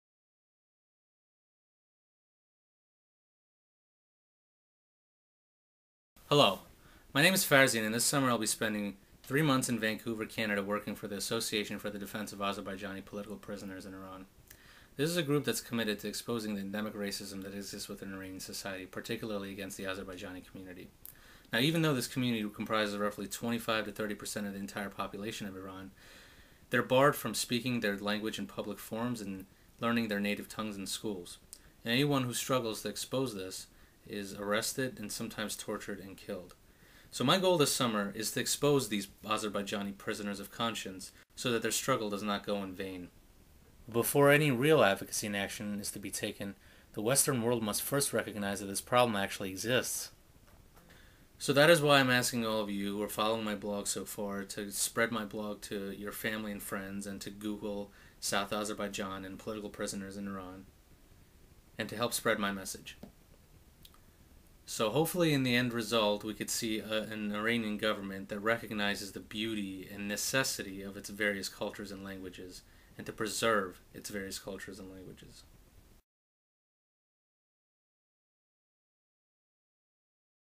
Peace Fellow Profile: Farzin and ADAPP
Farzin discusses his expectations for his Peace Fellowship in Vancouver with ADAPP.